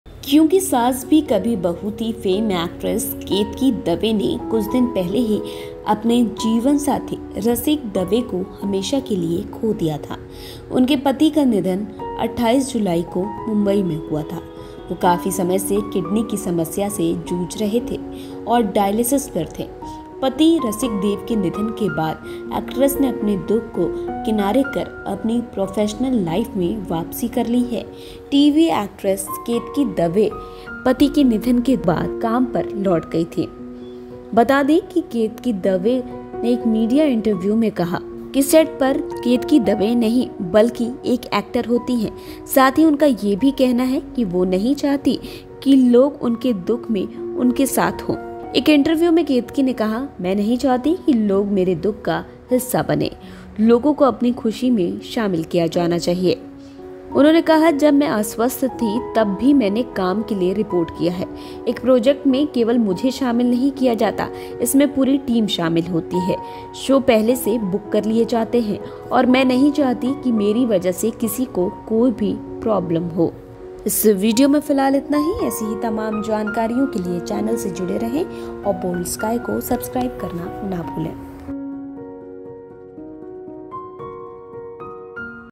Ketki Dave Husband Rasik Demise के 2 दिन बाद Shooting पर लौटीं,मेरे दुख में..। Boldsky*Entertainment
Kyunki Saas Bhi Kabhi Bahu Thi' fame actress Ketki Dave had lost her life partner Rasik Dave forever a few days back. Her husband demise on 28 July 2022 in Mumbai. He was suffering from kidney problem for a long time and was on dialysis. After the demise of husband Rasik Dave, the actress has put her sorrows aside and returned to her professional life.

क्योंकि सास भी कभी बहू थी’ फेम एक्ट्रेस केतकी दवे ने कुछ दिन पहले ही अपने जीवनसाथी रसिक दवेको हमेशा के लिए खो दिया था. उनके पति का निधन 28 जुलाई 2022 को मुंबई में हुआ था. वह काफी समय से किडनी की समस्या से जूझ रहे थे और डायलिसिस पर थे. पति रसिक दवे के निधन के बाद एक्ट्रेस ने अपने दुख को किनारे कर अपनी प्रोफेशनल लाइफ में वापसी कर ली है.